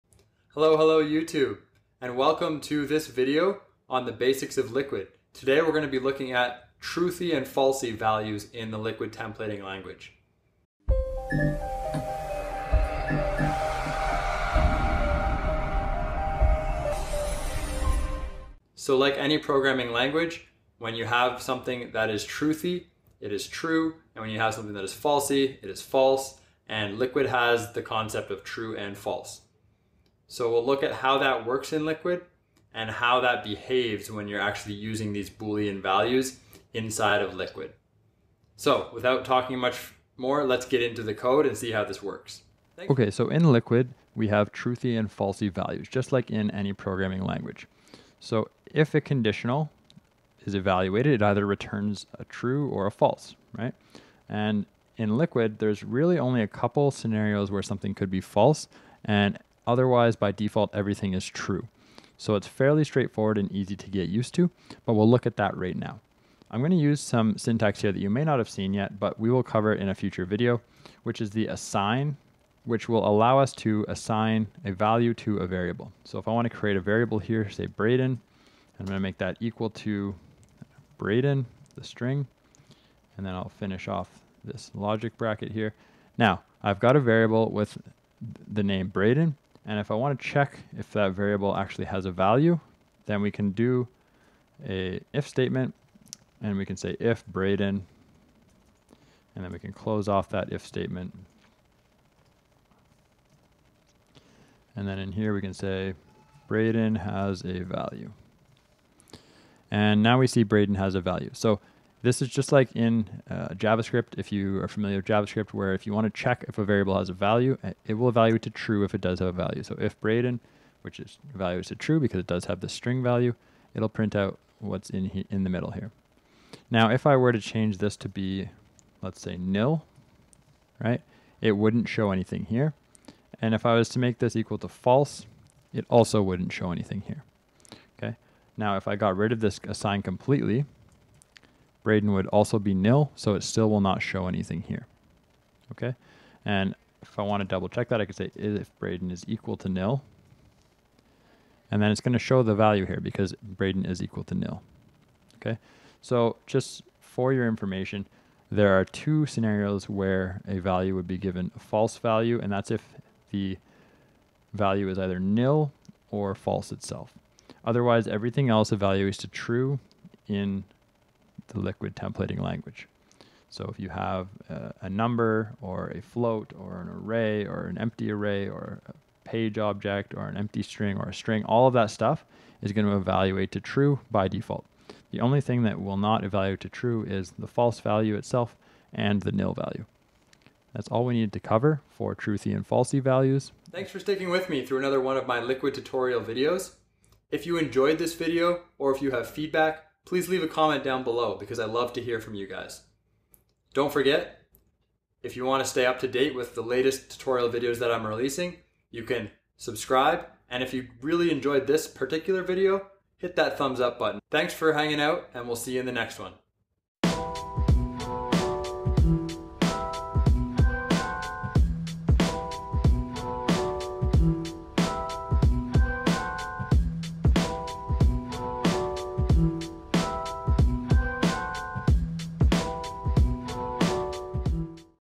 Shopify Liquid Basics: Truthy and Falsy
In this video we look at truthy and falsely values in the liquid templating language, how they are used and what we can do with them.